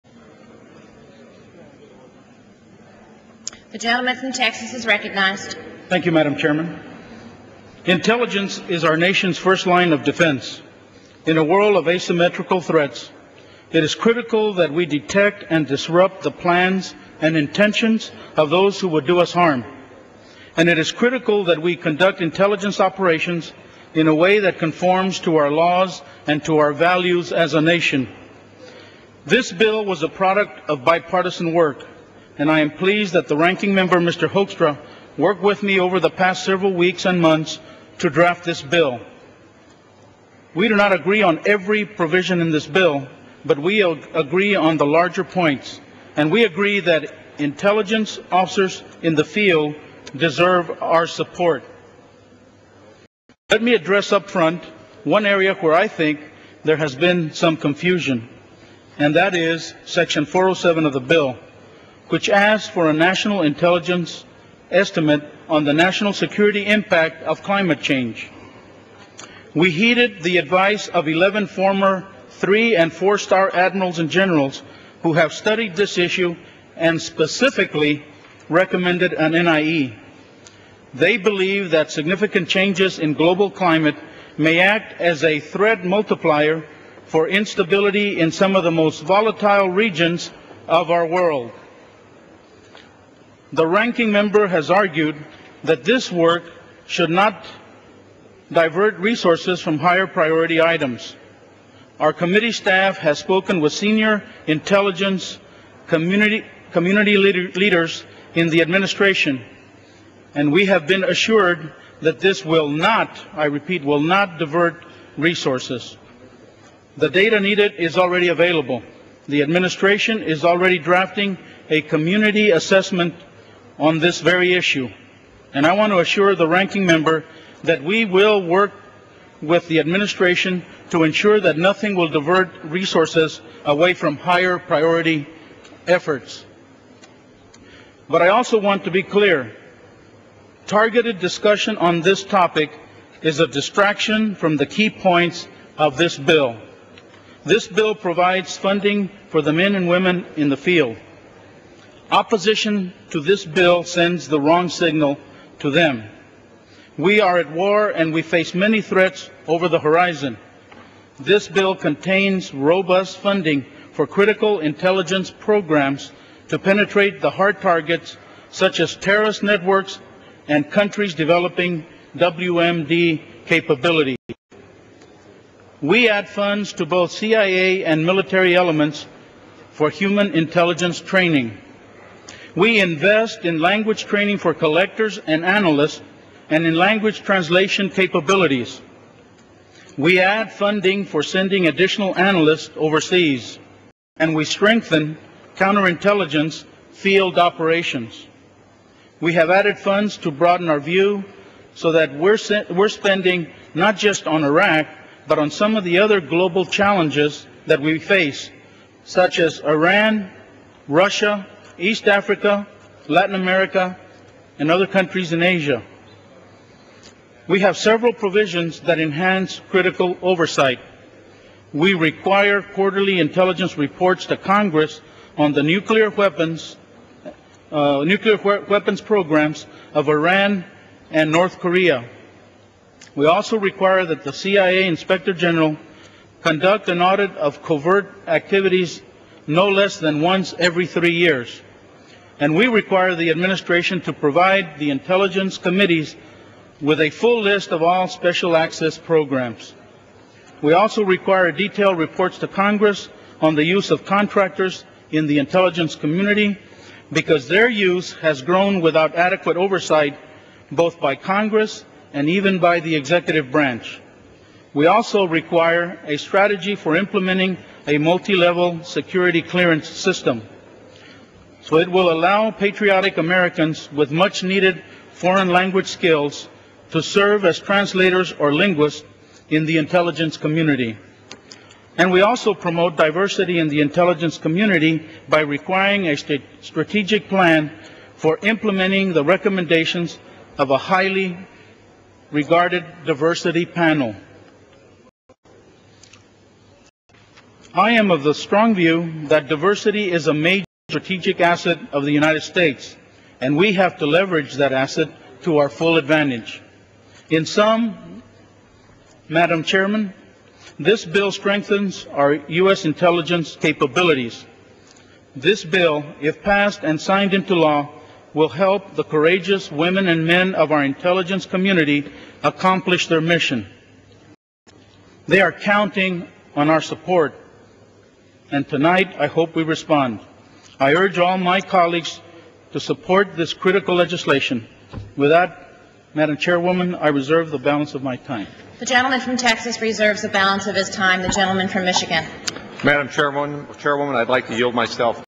Chairman Reyes on Intelligence Authorization
Tonight, the House is considering HR 2082, the FY 2008 Intelligence Authorization. The bill strengthens intelligence by adding funds to both CIA and military elements for Human Intelligence (HUMINT) training, investing in language training for intelligence collectors and analysts in language translation capabilities, adding funding for sending additional intelligence analysts overseas, and investing in strengthening counterintelligence field operations. The bill promotes efficiency and effectiveness in intelligence programs, by streamlining acquisition, trimming the fat from ineffective programs, eliminating redundant activities, and requiring greater strategic focus in some key areas.